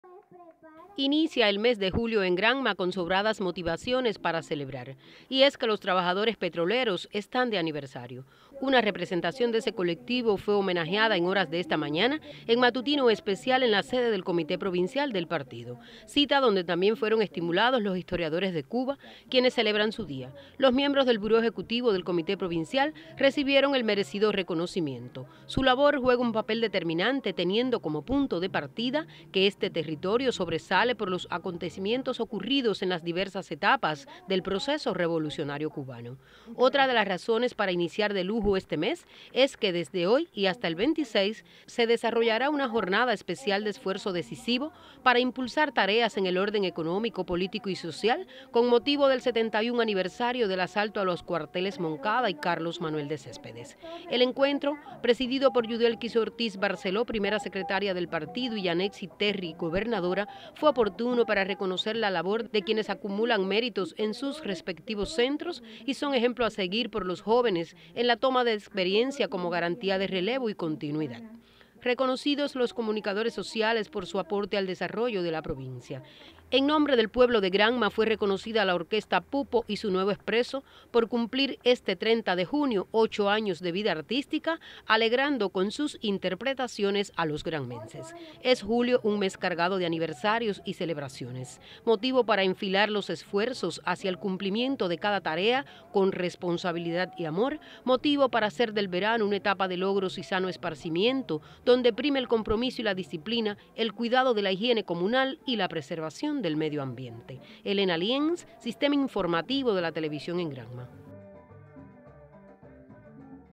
Labor del historiador es de vital importancia para el desarrollo del país
En horas de la mañana se desarrolló un matutino especial en la sede del Comité Provincial del Partido. En la cita se  reconoció la labor de los trabajadores petroleros, los comunicadores sociales y los historiadores de Cuba por celebrar aniversarios de constituidas sus respectivas organizaciones. El encuentro estuvo presidido por Yudelkis Ortiz Barceló, Primera Secretaria del Partido y Yanetsy Terry, Gobernadora.